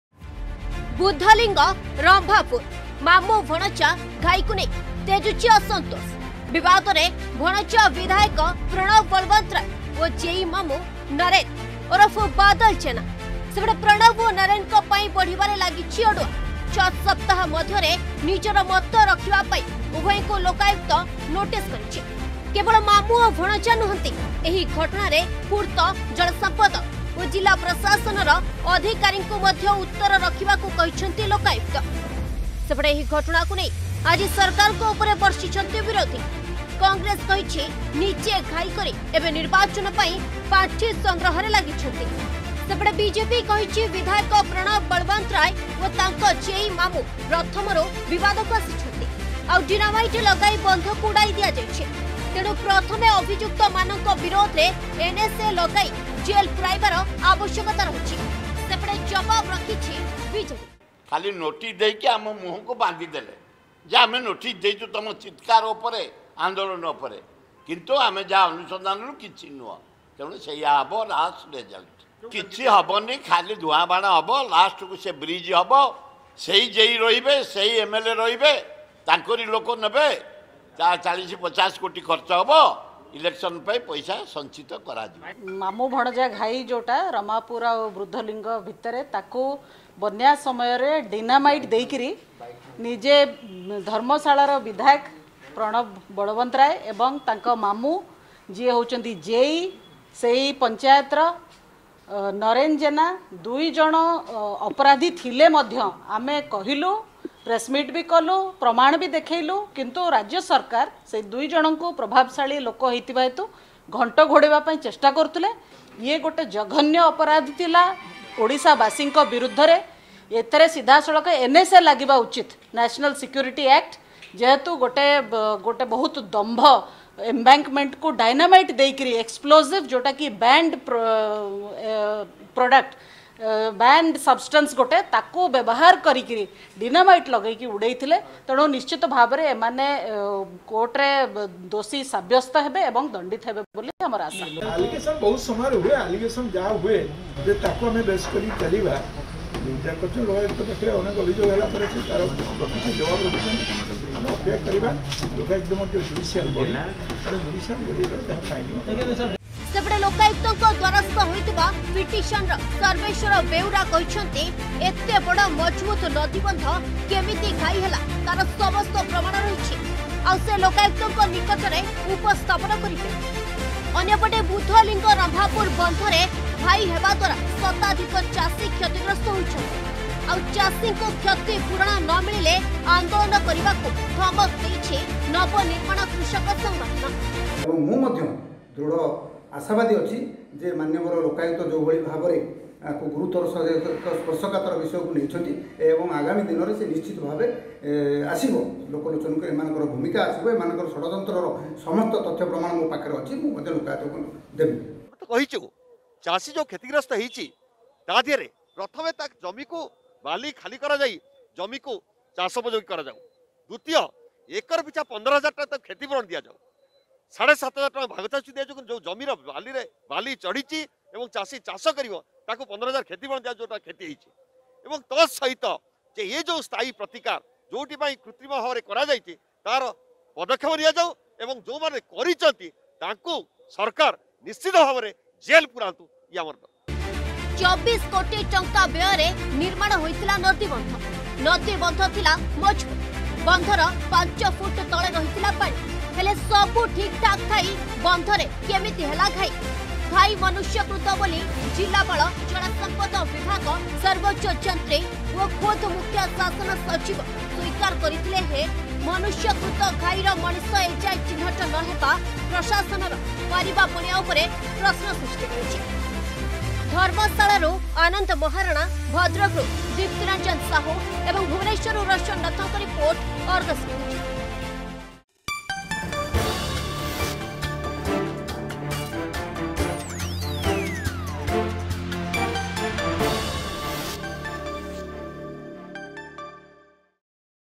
ମାମୁଁ-ଭଣଜା ଘାଇକୁ ନେଇ ତେଜୁଛି ଅସନ୍ତୋଷ
ବିଧାୟକ ପ୍ରଣବ ବଳବନ୍ତରାୟ ଓ ତାଙ୍କର ଜେଇ ମାମୁଁ ନରେନ୍ ଓରଫ ବାଦଲ ଜେନାଙ୍କ ପାଇଁ ବଢିଲା ଅଡୁଆ । ଲୋକାୟୁକ୍ତଙ୍କ ନୋଟିସ ପରେ ଏବେ ହରଡ଼ଘଣାରେ ମାମୁଁ ଓ ଭଣଜା । ବହୁଚର୍ଚ୍ଚିତ ବୁଦ୍ଧଲିଙ୍ଗ-ରମ୍ଭାପୁର ମାମୁଁ-ଭଣଜା ଘାଇକୁ ନେଇ ବିବାଦକୁ ଆସିଥିଲେ ଉଭୟ .